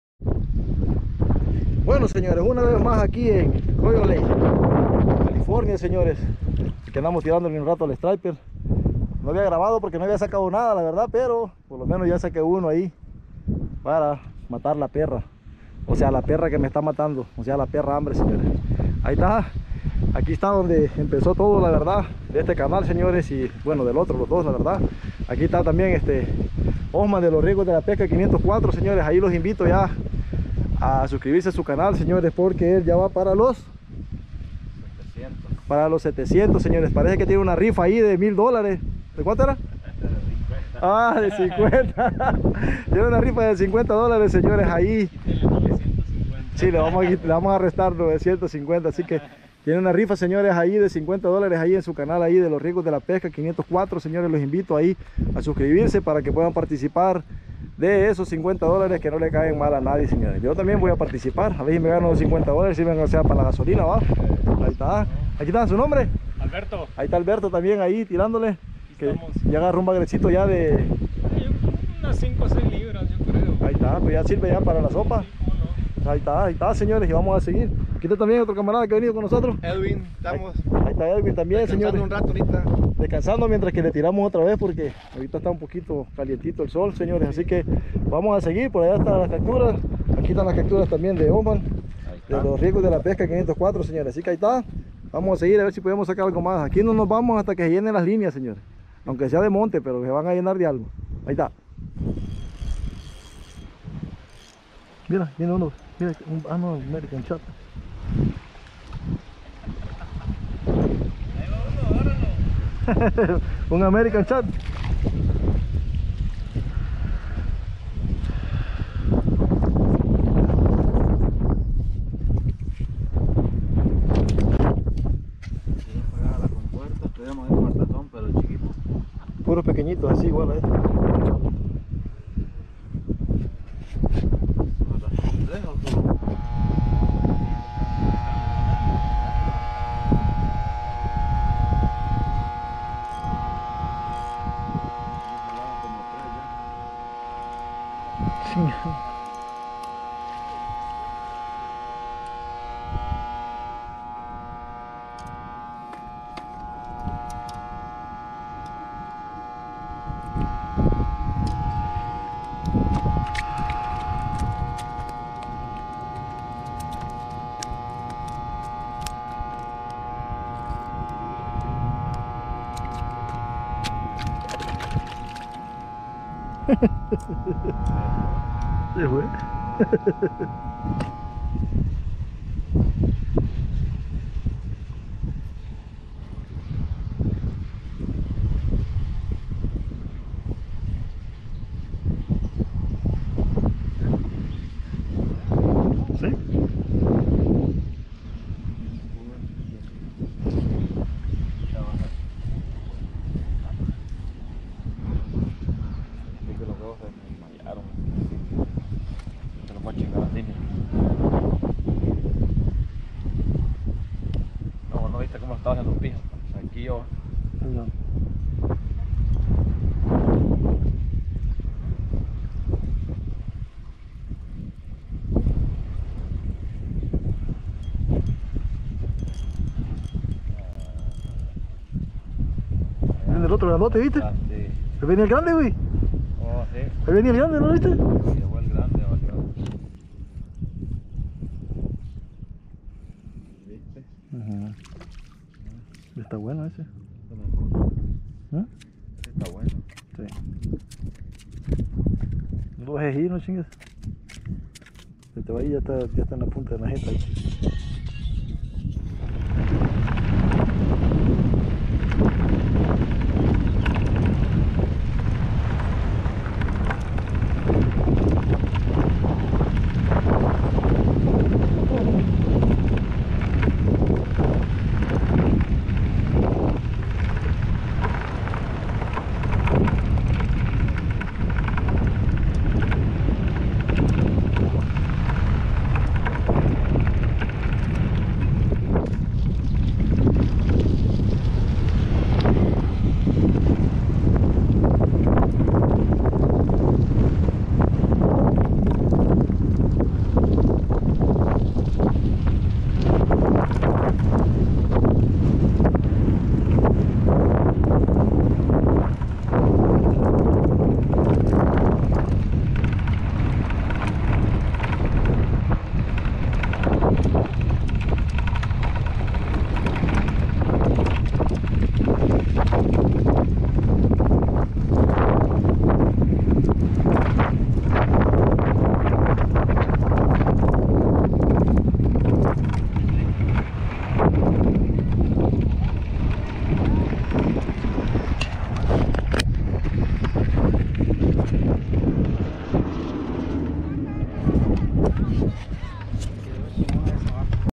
Pescando en Quail Lake ca
De pesca con @losriegosdelapesca y unos amigos